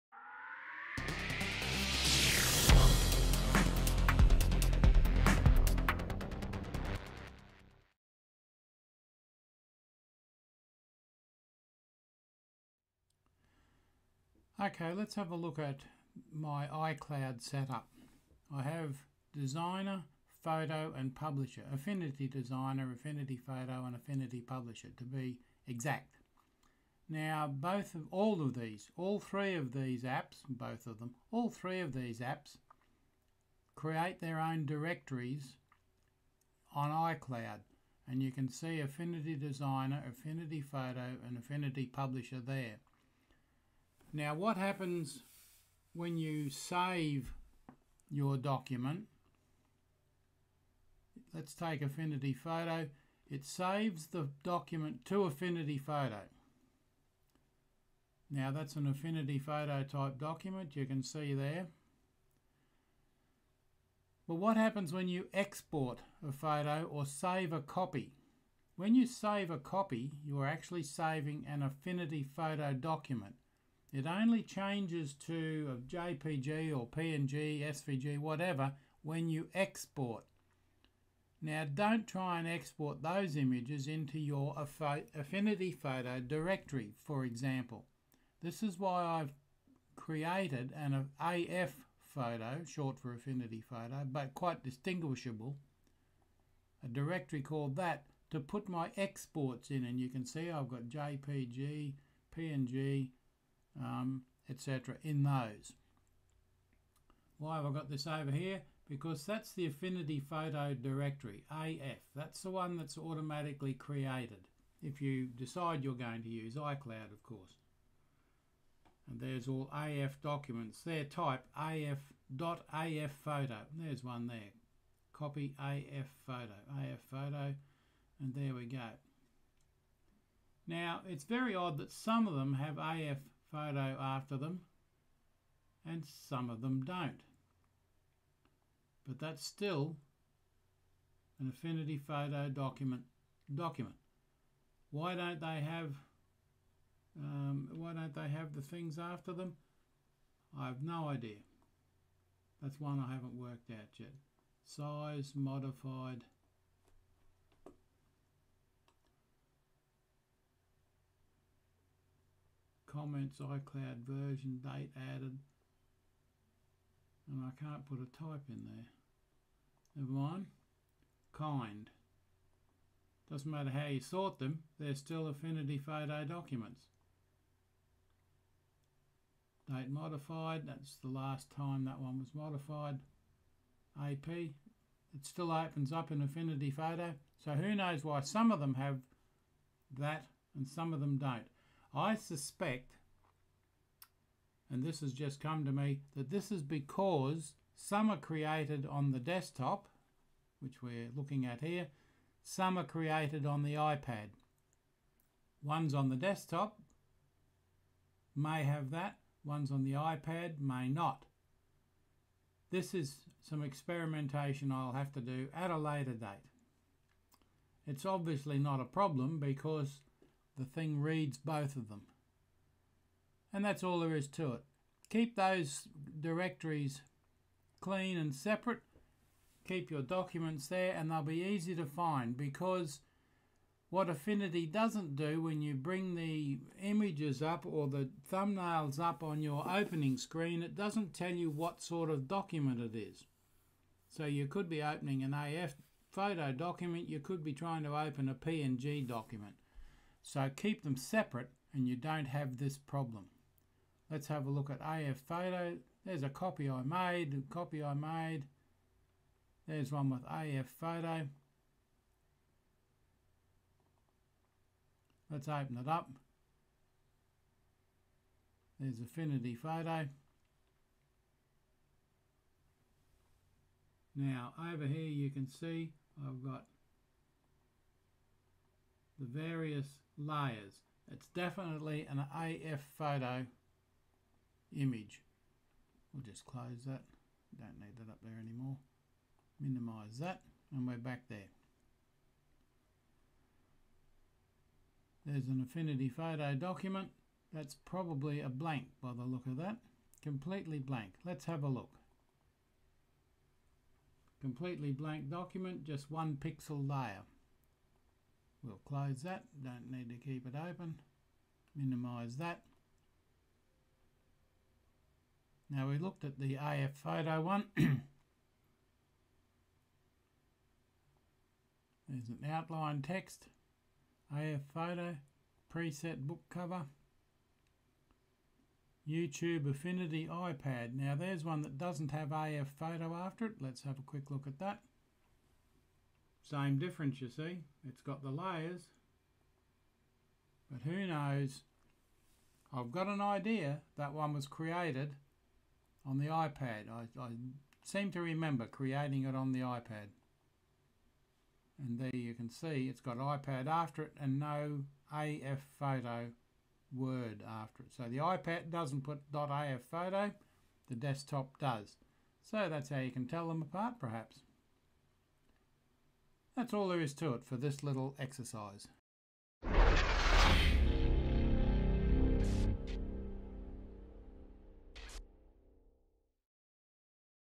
Affinity Folder Housekeeping
A brief look at the Affinity Folders on iCloud. Some the app creates, some you create. Creating your own folders allows you to keep your app generated images or projects entirely separate from the Exported images. Some people like housekeeping, some don't. Personally, I like to be able to find my work when I come back to it. Especially when your export has the same name as your working project.
I'm sure that if you don't actually have iCloud - and many people don't your mileage will be different. Enjoy.
If you use an iPad you will almost certainly have iCloud. Use it. Don't be a Luddite.
So - Here Be Dragons...
On your iCloud directory, you have a folder called
Affinity Photo.

Desktop app.
* When you create a new project on the DESKTOP, and save it. Just "Save" not Save A Copy, you would by default save it as an .afphoto file to this Affinity Photo folder.

iPad app
* When you create a new project on the iPad and save it. Just "Save" not "Save A Copy", you would by default save it as an .afphoto file to the PHOTO folder "On My iPad". But Not on the iCloud!

ok, are you with me?  So there's more.
* If you are working on the iPad, and want to take your project and save it where you can continue on your DESKTOP to work on it, you  Can't  "Save As" to your Desktop's native Affinity Photo folder. It's locked to you. You must create a folder of your own on the iCloud Drive and retrieve your file to the desktop from there. In my case, I called the folder af-photo. In that folder, you can have a mixture of exported files like PNG, JPG, afphoto etc. Anything you like. It's not exclusive in other words.
Ok - still with me?
To recap.
- Desktop. Native folder "Affinity Photo" and should hold only .afphoto files. Why? Because you can have a file called myimage.jpg and myimage.afphoto and they both show up in the folder with only the file name. You guess which one is the afphoto one. So save your exports etc somewhere else.
- iPad. Native folder "Photo" on the iPad, and should hold only .afphoto files. Store your "Save As" files somewhere else. For the same reasons as for the Desktop.